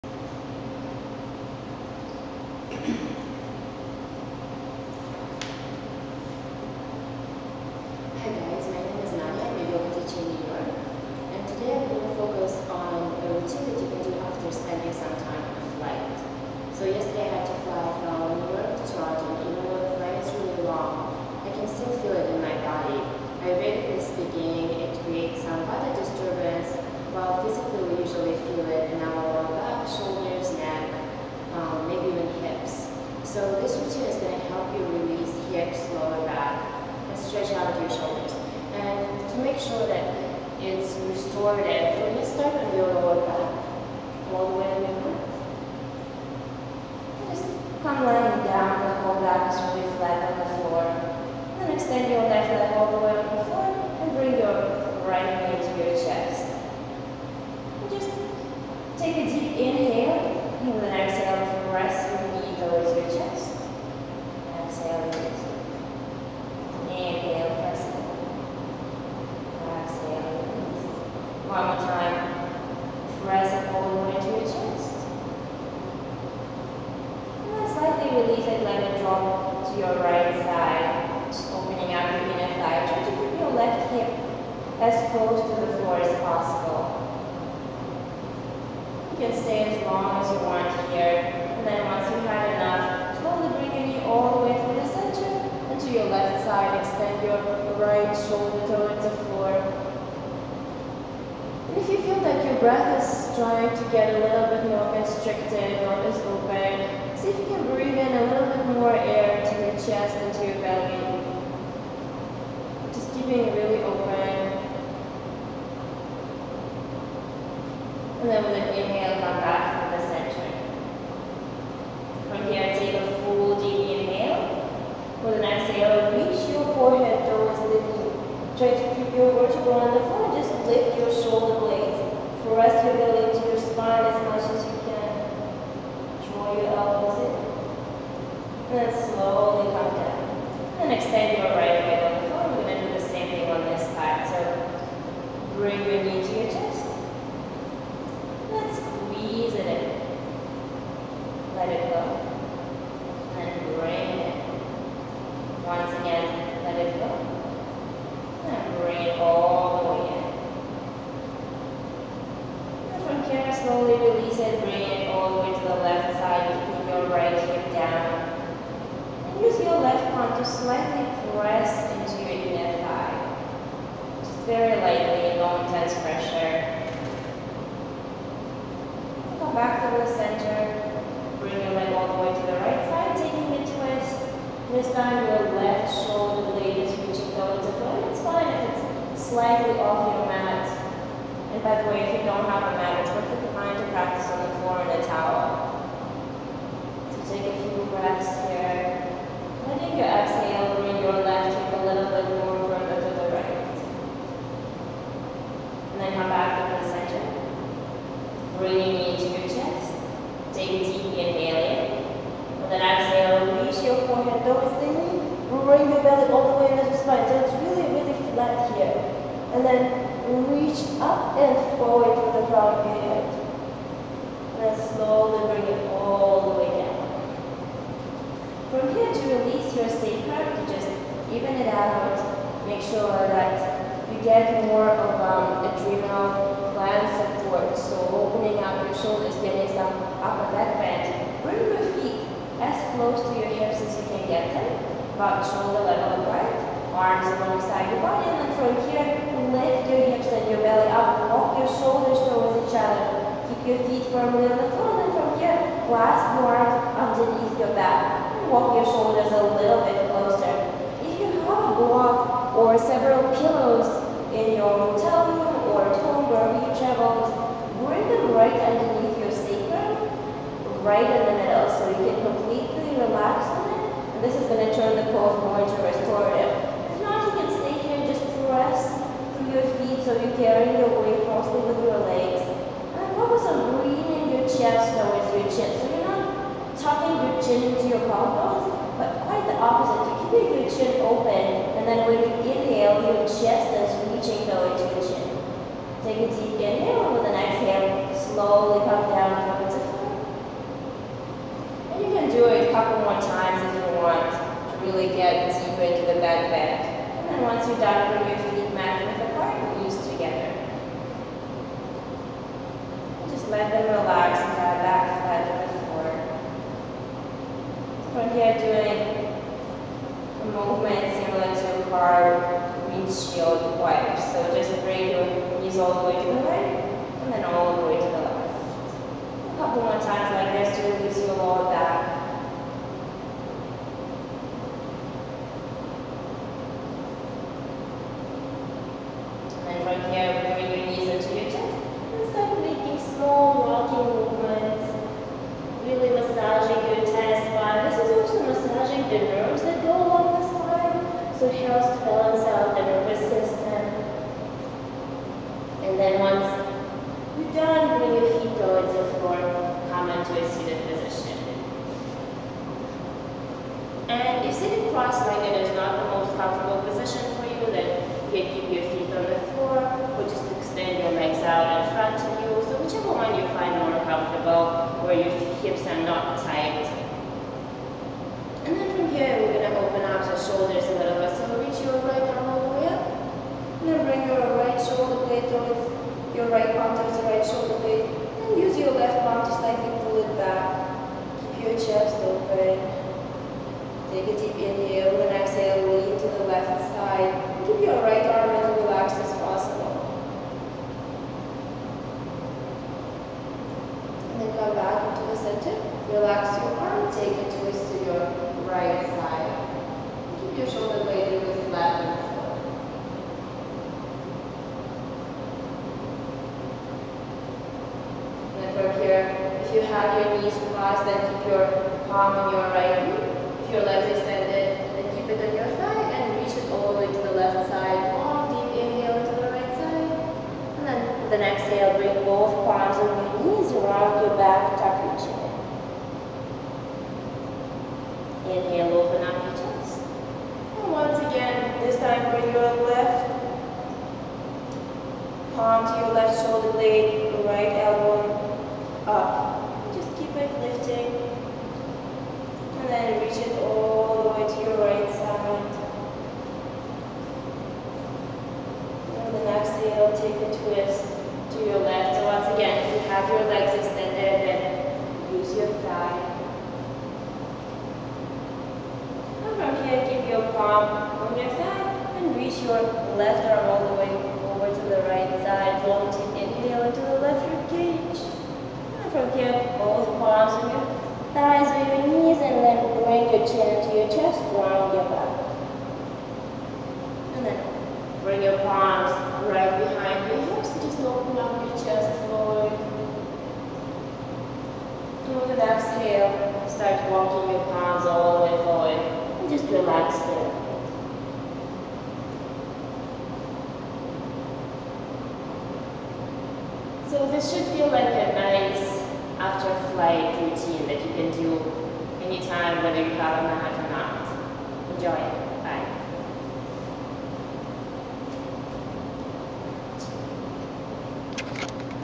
After Flight Yoga Stretch
Do this yoga routine to relieve discomfort in your lower back, hips, and shoulders after a long flight